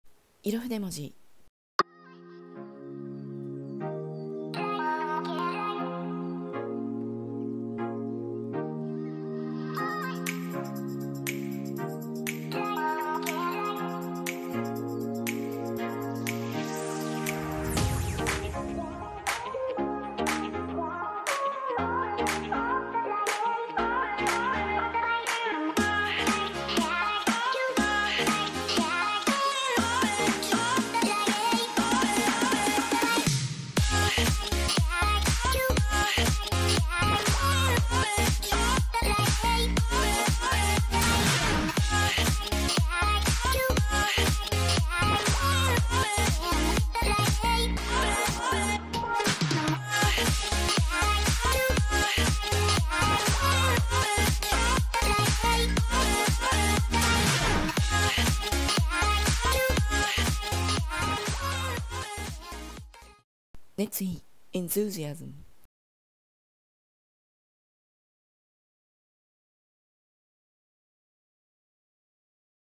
色筆文字～Enthusiasm😄
色と筆文字のコラボレーション🖌️✨

今日のメッセージ😄

『色』は心を映し出す鏡✨

『筆文字』は今の自分へのメッセージ✨

お好きな文字を、しらさぎの鳥の羽の筆と

麻墨を使ってお書きします😊

麻墨は、魔祓い、清めとされるため
文字により身を守り、場を清められます✨